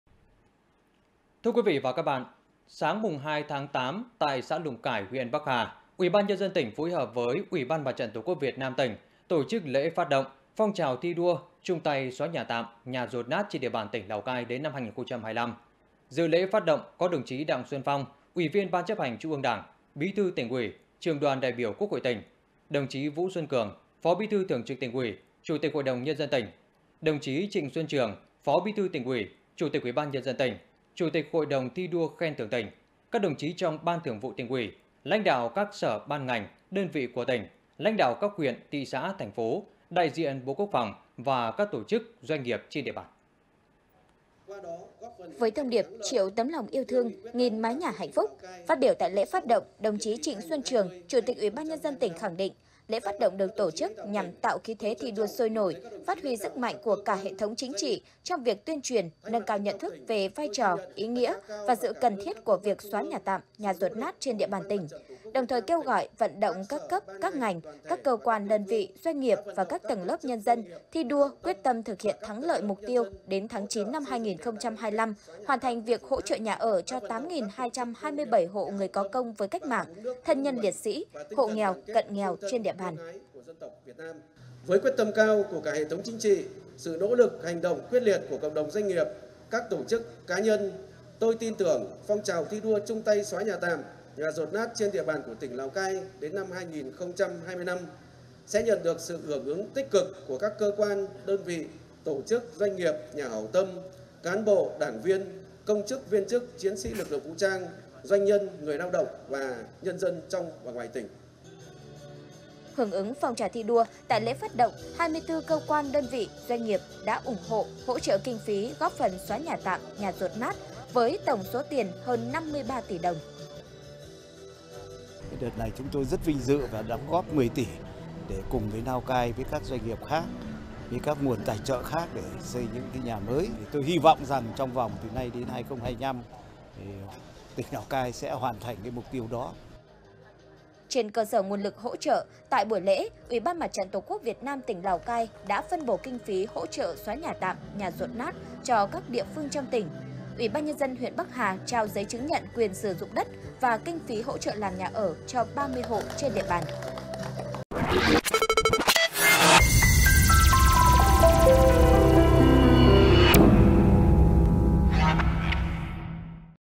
Hơn 53 tỷ đồng ủng hộ tại lễ phát động phong trào thi đua "Chung tay xóa nhà tạm, nhà dột nát"| THLC
Laocaitv.vn - Sáng 02/8, tại xã Lùng Cải, huyện Bắc Hà, UBND tỉnh phối hợp với Ủy ban Mặt trận Tổ quốc Việt Nam tỉnh tổ chức Lễ phát động Phong trào thi đua “Chung tay xóa nhà tạm, nhà dột nát trên địa bàn tỉnh Lào Cai đến năm 2025”. Dự Lễ phát động có đồng chí Đặng Xuân Phong, Uỷ viên Ban Chấp hành Trung ương Đảng, Bí thư Tỉnh uỷ, Trưởng đoàn đại biểu Quốc hội tỉnh; đồng chí Vũ Xuân Cường, Phó Bí thư Thường trực Tỉnh uỷ, Chủ tịch HĐND tỉnh; đồng chí Trịnh Xuân Trường, Phó Bí thư Tỉnh uỷ, Chủ tịch UBND tỉnh, Chủ tịch Hội đồng Thi đua - Khen thưởng tỉnh; các đồng chí trong Ban Thường vụ Tỉnh uỷ; lãnh đạo các sở, ban, ngành, đơn vị của tỉnh; lãnh đạo các huyện, thị xã, thành phố; đại diện Bộ Quốc phòng và các tổ chức, doanh nghiệp trên địa bàn.
Đài Phát thanh - Truyền hình Lào Cai